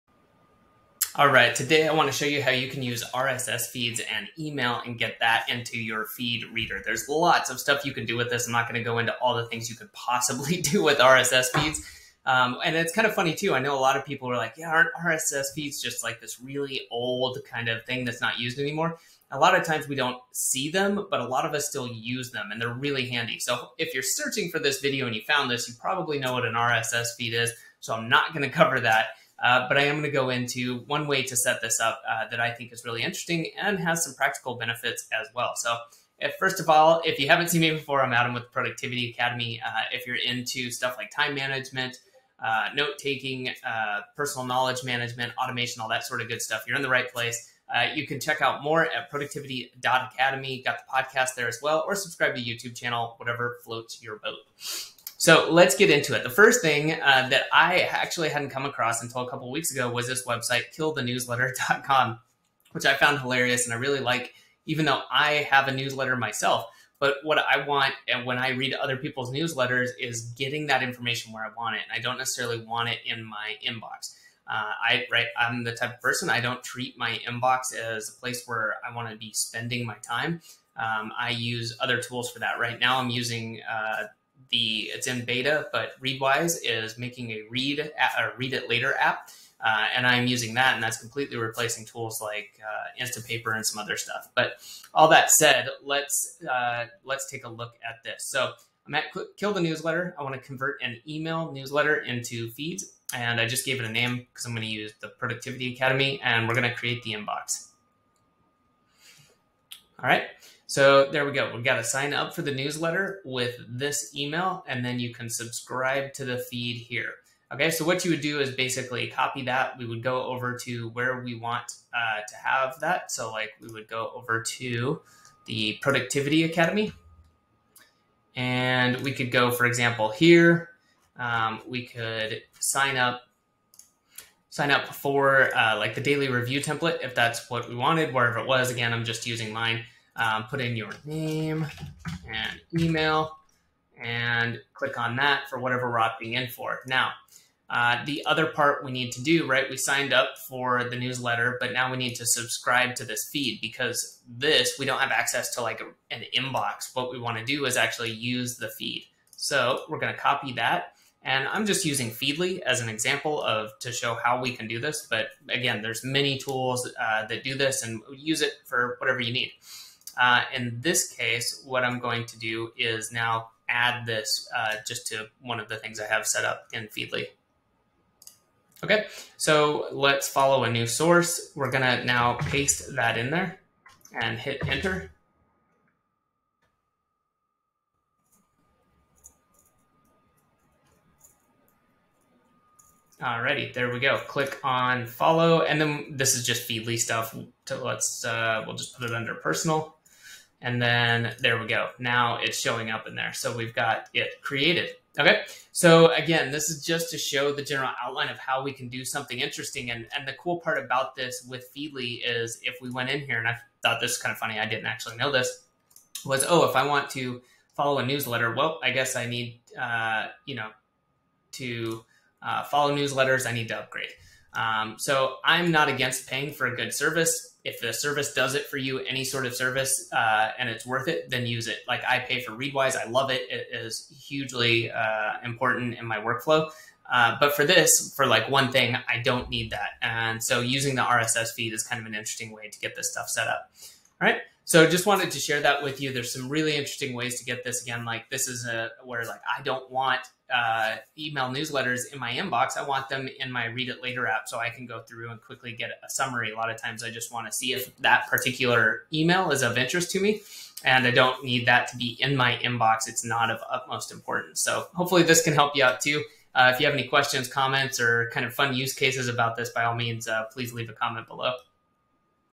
How To Get An RSS Feed For Emails
What can you do if you want an RSS feed for an email like a particular newsletter? Good news - here's one way that you can get an RSS feed and keep your inbox from getting too cluttered.

Also included in this video is one way that you can then use that RSS feed and import it into a tool, in this case, Feedly.